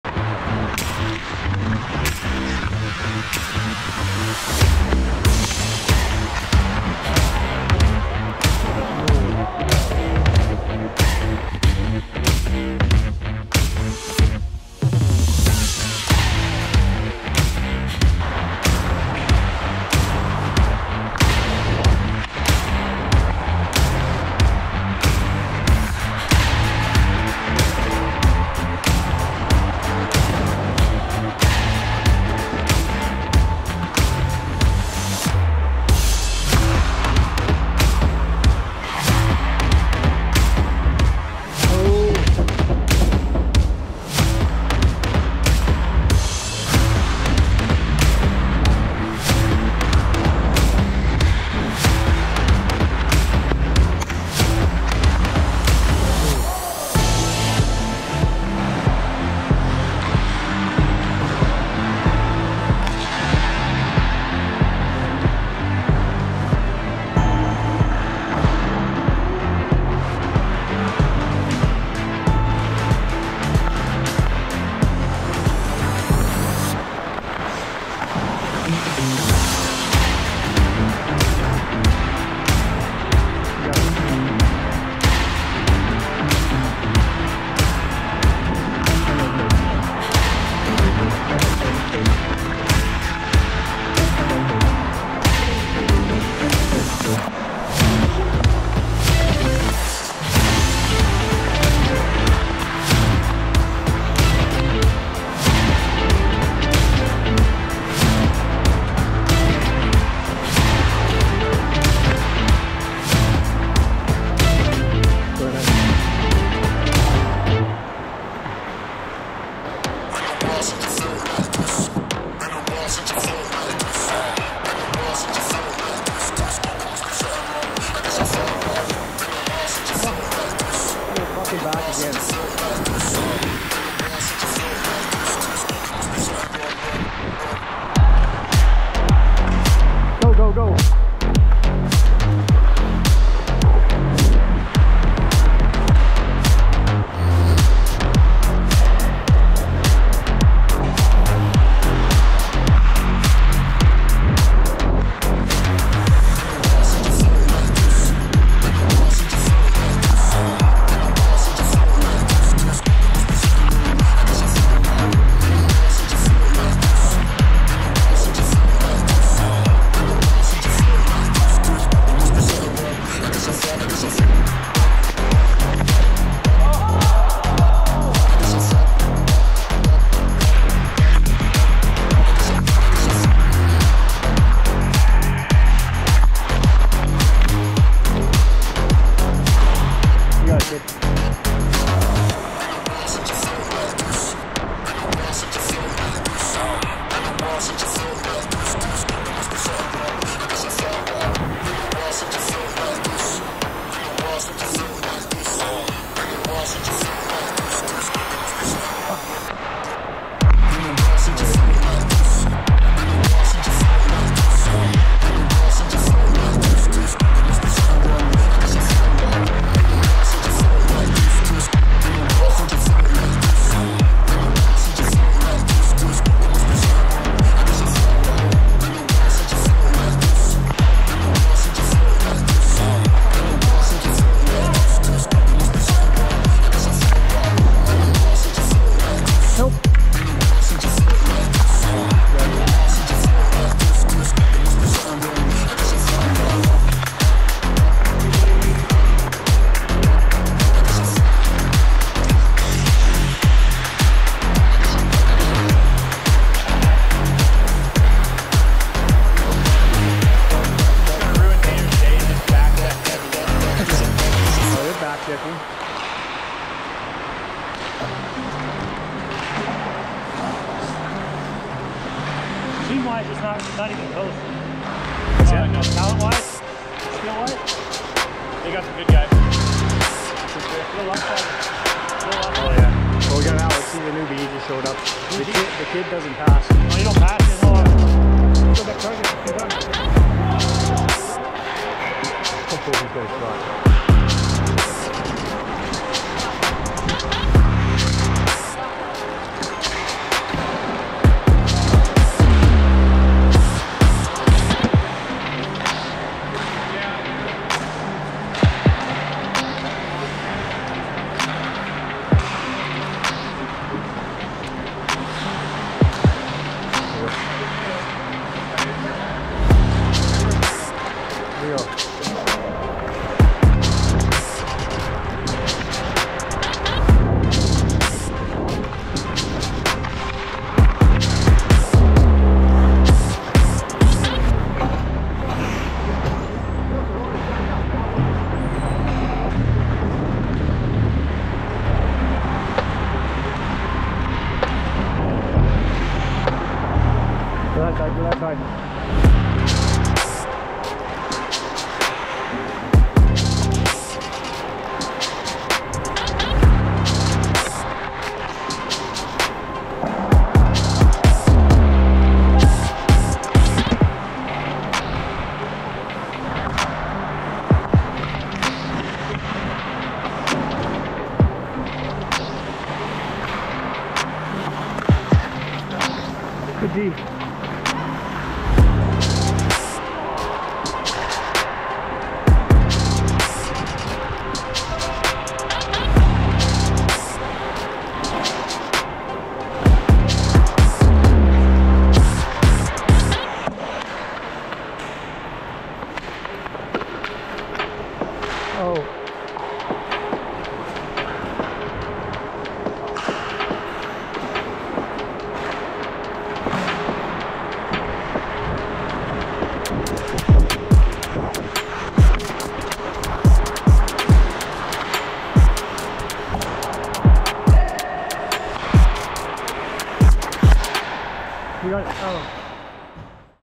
Summer 2023 Shinny - GoPro Hockey
Getting more of the footage I've gotten over the past year edited and posted.

GoPro on the helmet makes for a cool perspective that I might have to go back to on occasion.

@prickly_mitts_hockey on IG and TikTok